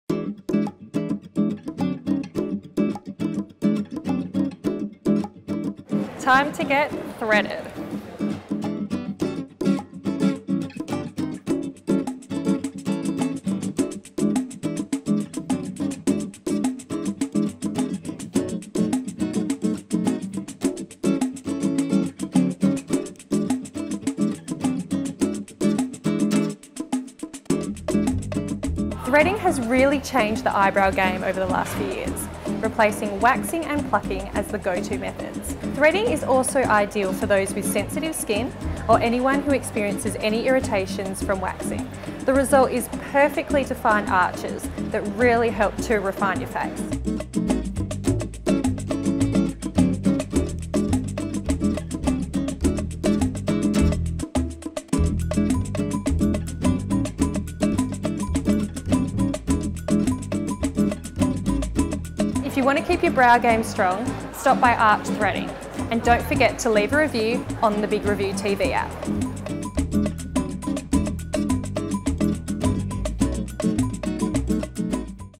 Arch Threading a Beauty Salon in Perth offering Eyebrow Threading and Facial
Arch Threading offers a range of beauty services including eyebrow threading, facial, eyelash extensions, and more.

Location: Shop 1104A 1382 Westfield Carousel located in Woolworth side mall Perth WA Australia 6107